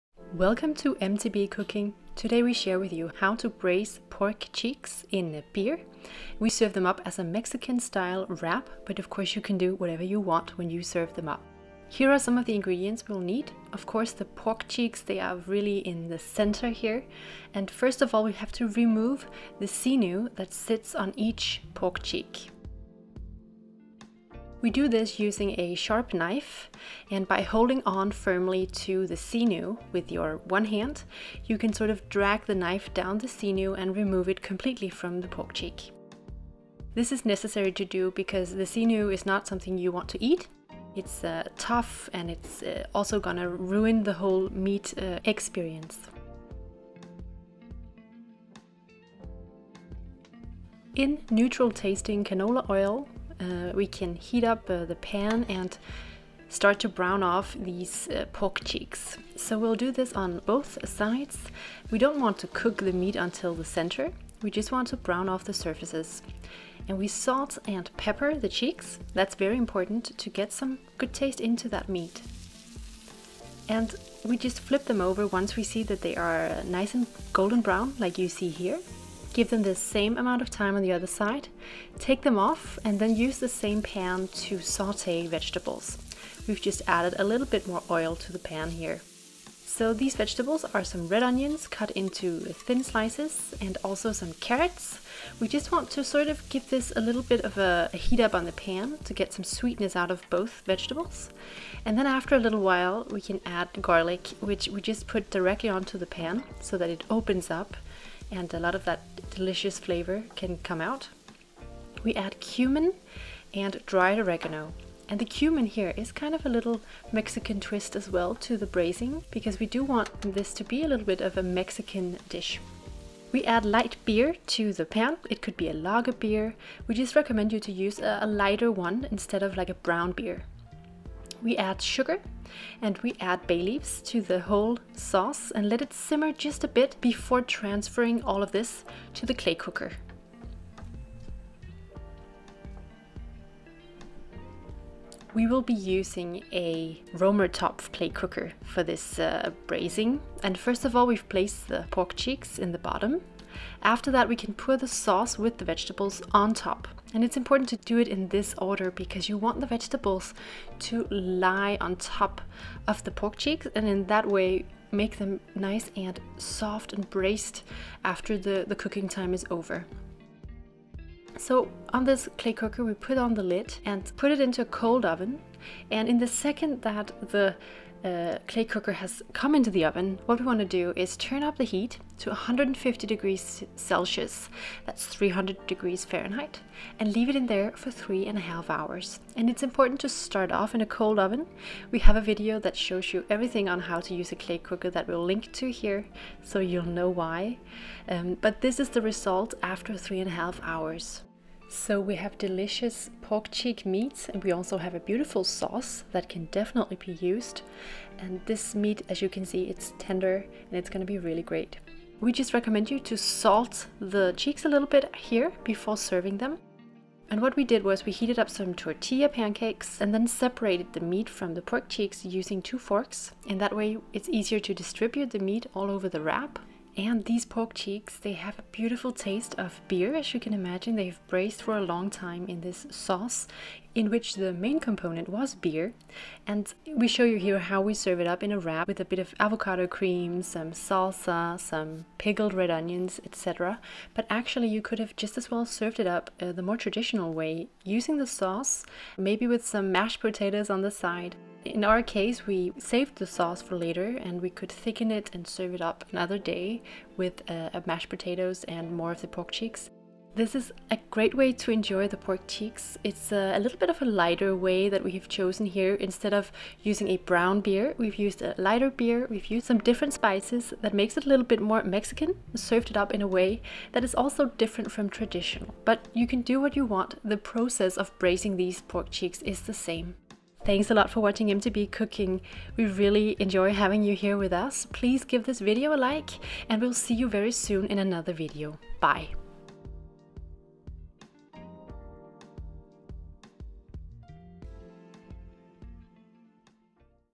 Pork Cheeks Braised in Beer in Römertopf / Clay Cooker - Recipe # 135
A great recipe for pork cheeks braised in a blond beer. We serve them up Mexican style in a tortilla wrap! But you could just as well serve them with the cooking sauce and mashed potatoes. Easy to make, this dish will only need some of your time...

Ingredients for 8 people (large portion, which can easily be halved)
2 kg / 4 pounds of pork cheeks
500 ml / 16.9 fl. oz light beer (e.g. Lager)
5 cloves of garlic
4 red onions,
1 tbsp of cumin powder
2 tsp of dried oregano
1 tsp of cane sugar
2 bay leaves
a bit of chili (optional)
salt & pepper according to taste, but don't be cheap!

Serve with: wheat tortilla pancakes, guacamole / avocado cream, cherry tomato salsa, fresh cilantro / coriander & tortilla chips (of corn)

Cook:
In oven at 150 deg. C / 300 deg. F for 3.5 hours

Enjoy & bon appétit ! :-)